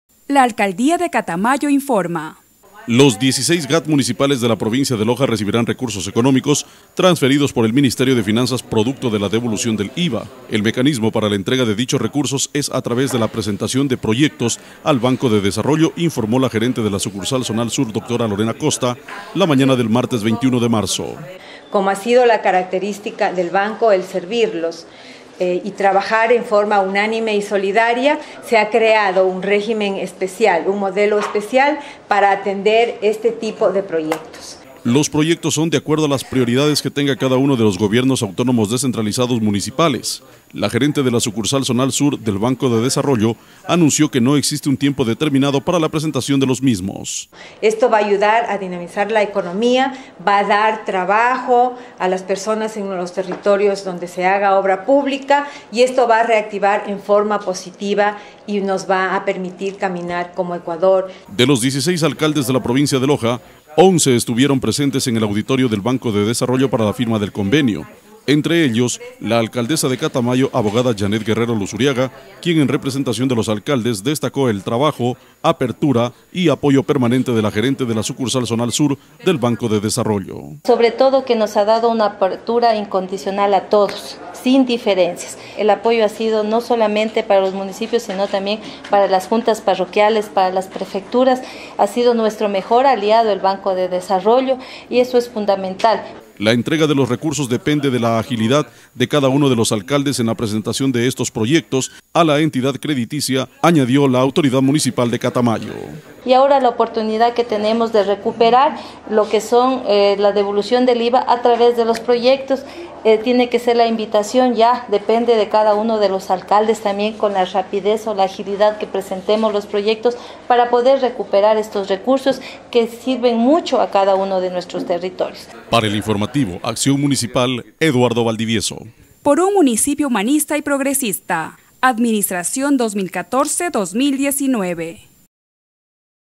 Gad municipales recibirán recursos por devolución del I.V.A
Los dieciséis Gad municipales de la provincia de Loja, recibirán recursos económicos, transferidos por el Ministerio de Finanzas producto de la devolución del I.V.A,  el mecanismo para la entrega de dichos recursos, es a través de la presentación de proyectos al Banco de Desarrollo, informó la Gerente de la Sucursal Zonal Sur Doctora Lorena Costa, la mañana del martes 21 de marzo.
Los proyectos son de acuerdo a las prioridades que tenga cada uno de los gobiernos autónomos descentralizados municipales, la Gerente de la Sucursal Zonal Sur del Banco de Desarrollo, anunció que no existe un tiempo determinado para la presentación de los mismos.
De los dieciséis alcaldes de la provincia de Loja, once estuvieron presentes en el auditorio del Banco de Desarrollo para la firma del convenio, entre ellos la Alcaldesa de Catamayo Abogada Janet Guerrero Luzuriaga, quien en representación de los alcaldes, destacó el trabajo, apertura y apoyo permanente de la Gerente de la Sucursal Zonal Sur del Banco de Desarrollo.
La entrega de los recursos depende de la agilidad de cada uno de los alcaldes, en la presentación de los proyectos a la entidad crediticia, añadió la autoridad municipal de Catamayo.